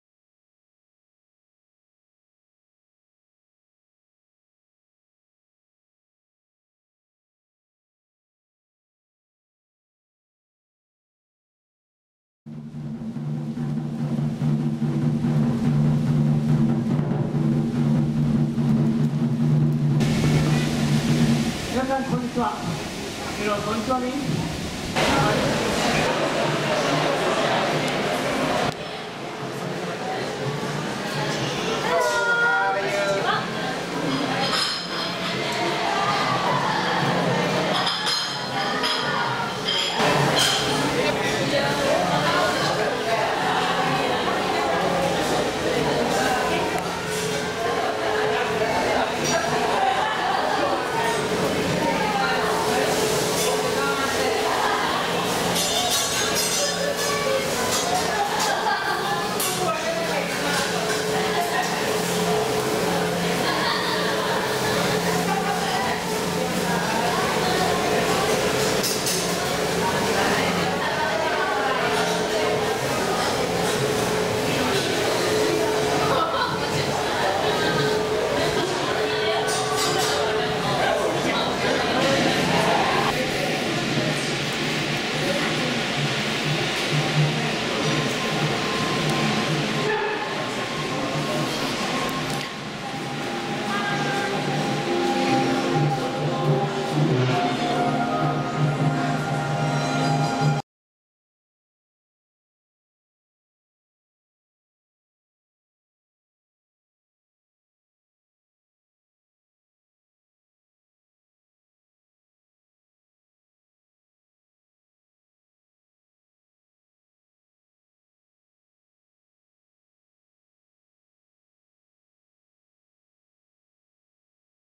Teppan Edo / Tokyo Dining, Epcot, Walt Disney World
Teppan Edo / Tokyo Dining in Epcot at Walt Disney World Resort (HD 1080p) — SKIP the LINES, with tips, apps and real tickets